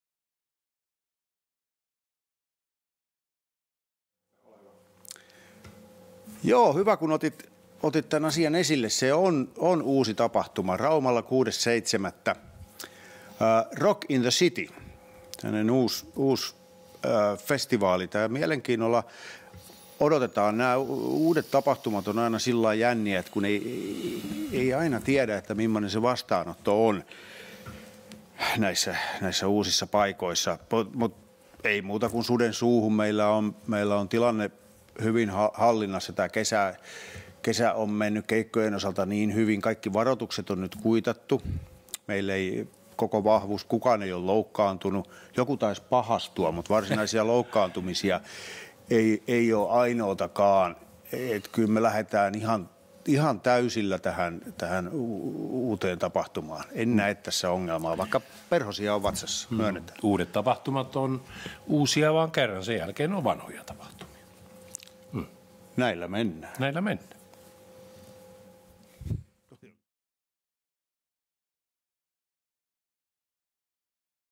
Rock in the City, Rauma, 6. 7. 2019.
Eppu Normaali Raumalla, 6.7.2019. Oiva tilaisuus olla oiva.